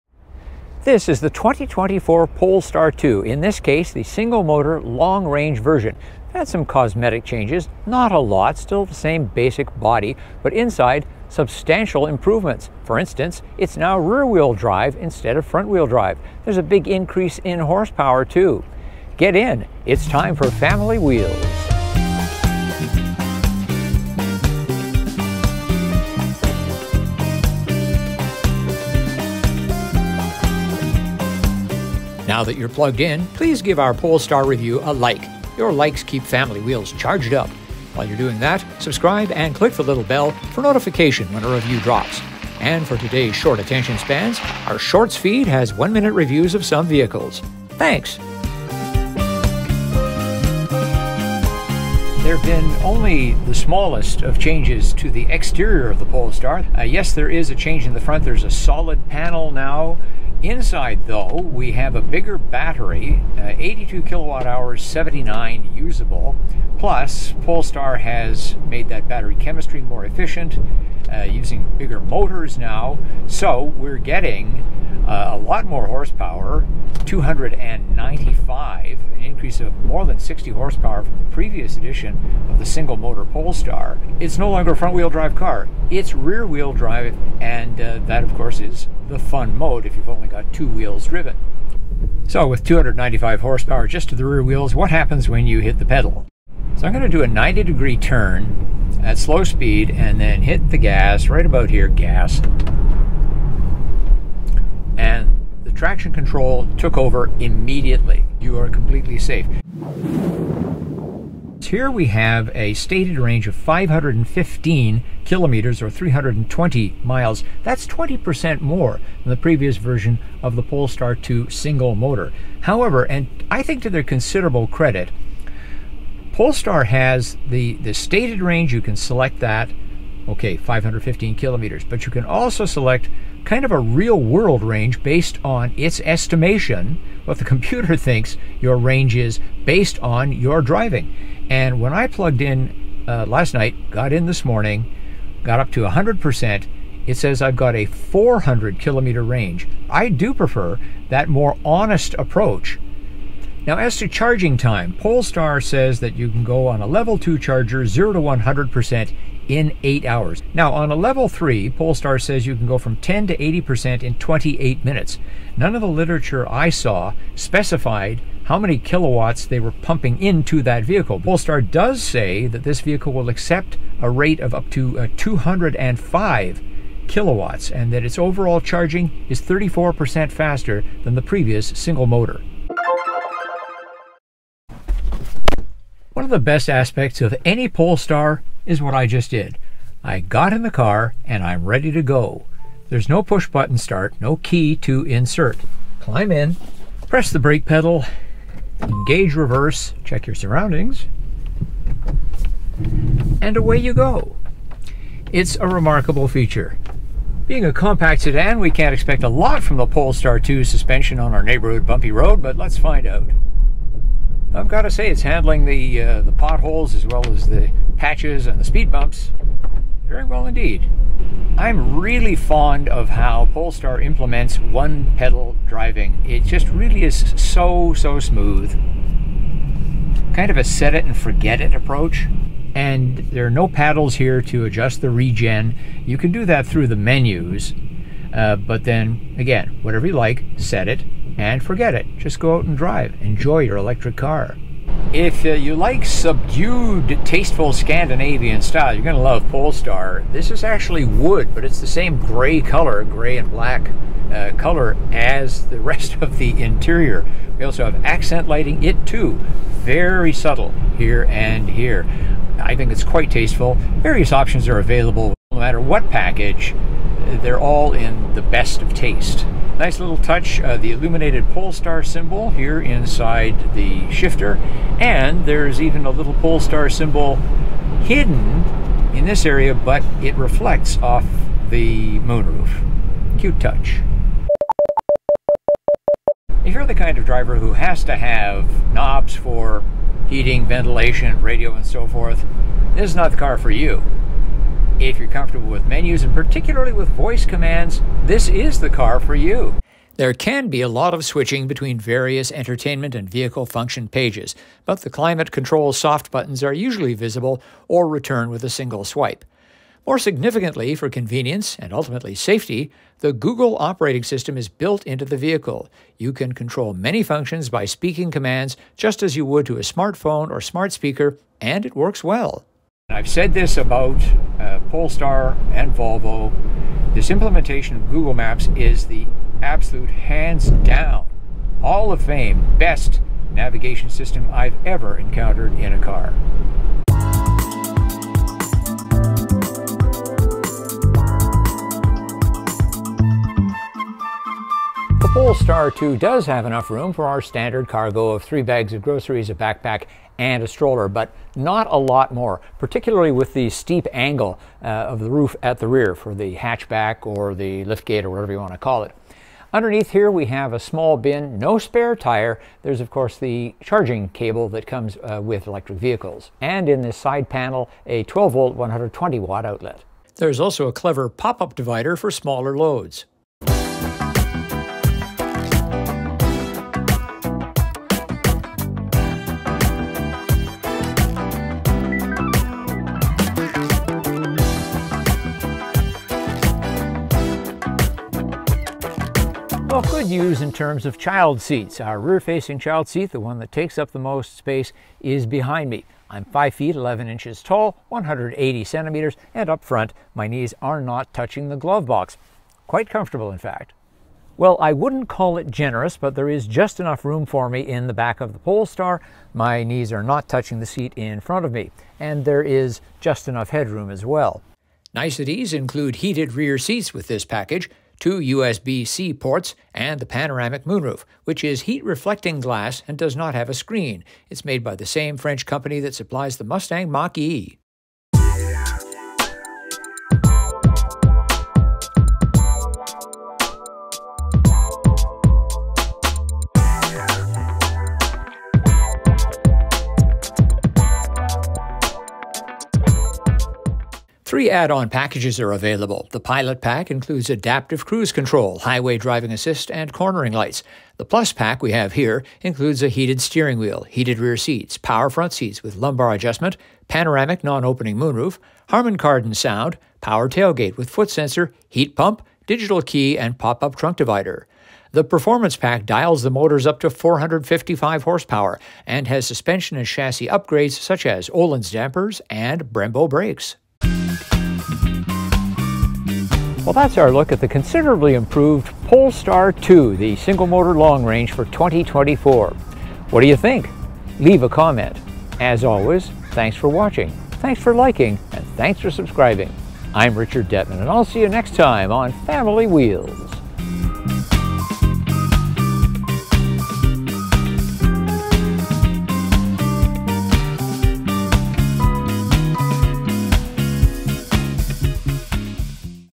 '24 Polestar 2: substantial improvements
The Polestar 2 Long range Single motor looks little different than its predecessor but has an improved drivetrain. It's now rear-wheel drive, has more horsepower, greater range and faster charging.